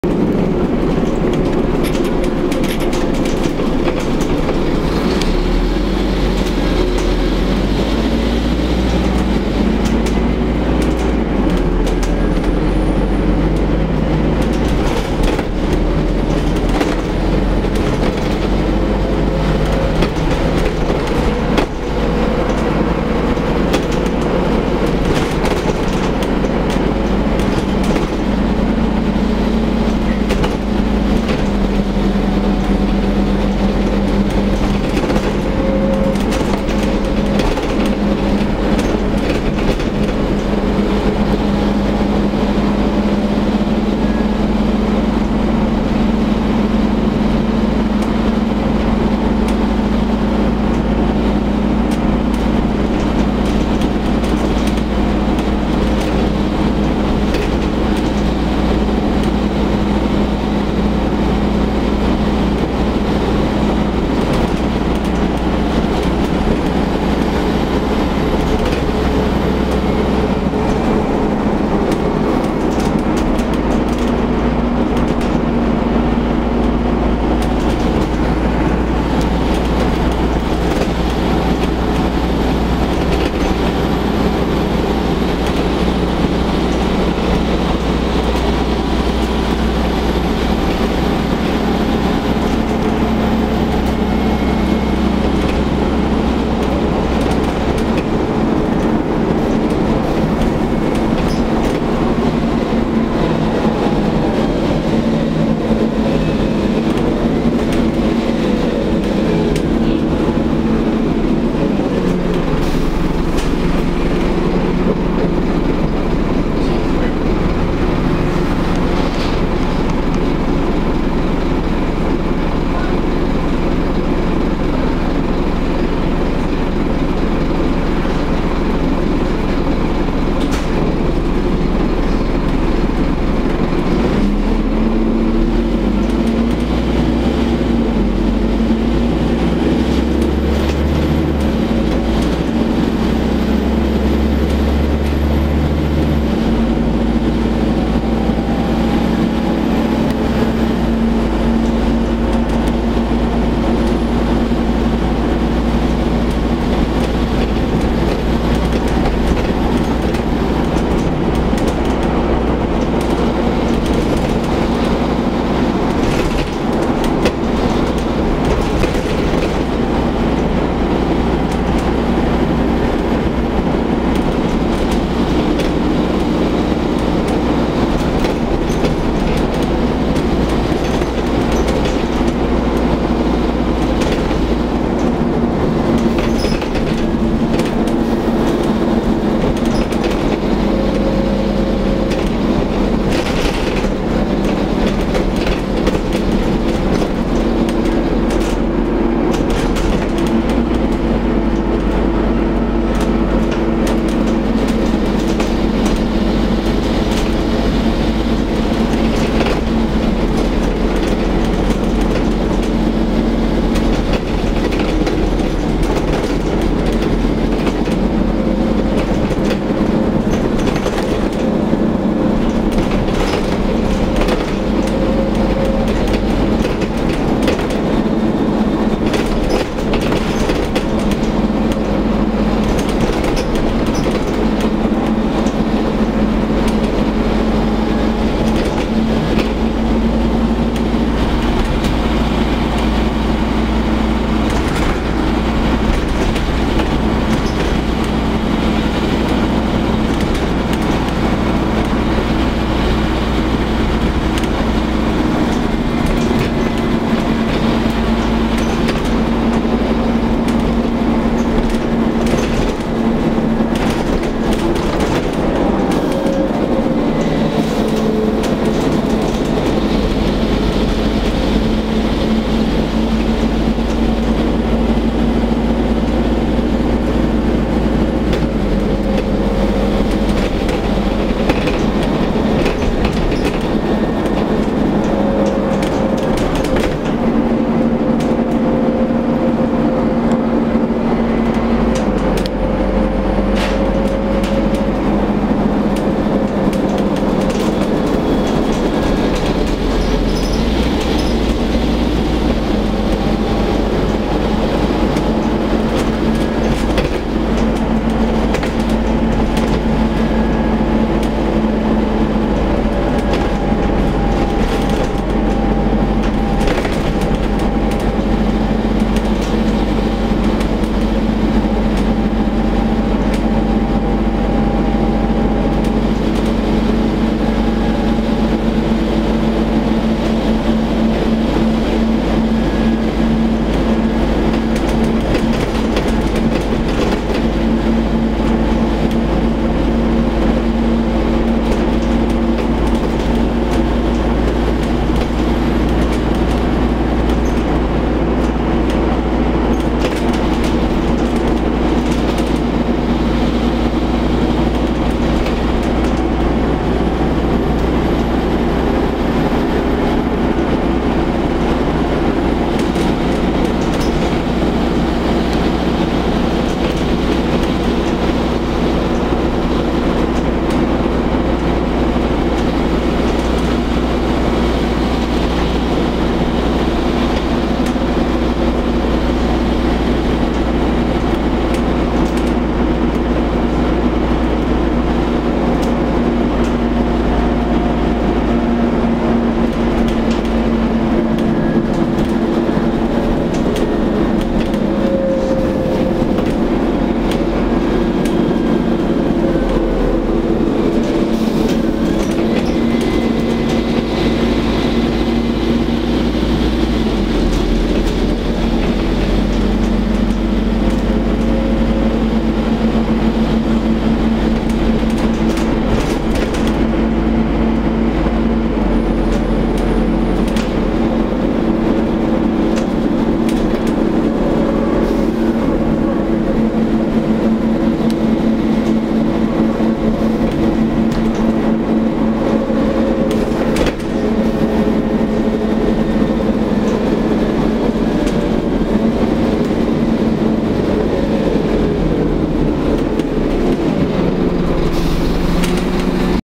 Stagecoach West 15450 MX08GHV
Stagecoach West Scania N230UD/Alexander Dennis Enviro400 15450 MX08GHV works route 33 to Gloucester.